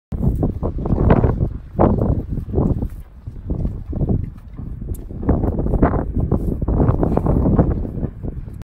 April enjoying her steamed hay
It's bitter cold this morning. About 15 degrees. April is enjoying her steamed hay pulled from my Haygain Steamer. - If you decide to order one get a discount!